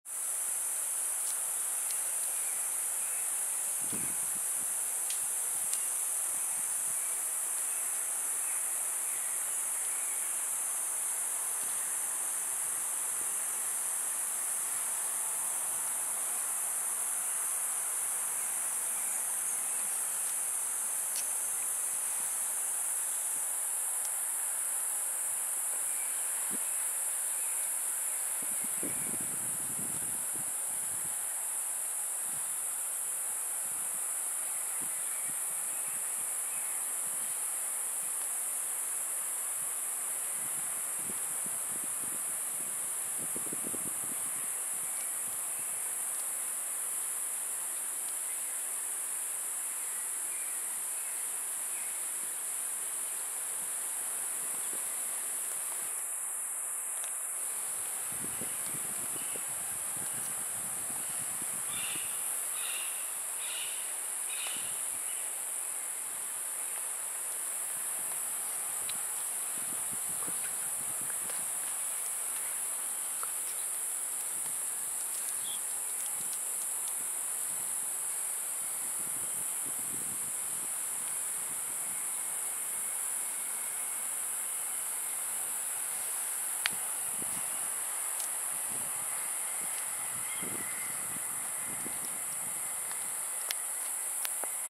Cute, curious sloth
This sloth gets very interested in a camera and tries to discover it, but he is a little bit shy too, so he hides for a while, but curiosity won.
Rodrigo is about 1,5 year old orphan sloth. He was found near the road, when he was about 8 months old and he was brought to Jaguar Rescue Cenetr in Puerto Viejo de Limon, Costa Rica. His mum was never found.